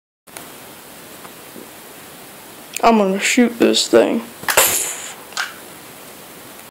Kid shoots tv with bb gun
This kid rages and shoots tv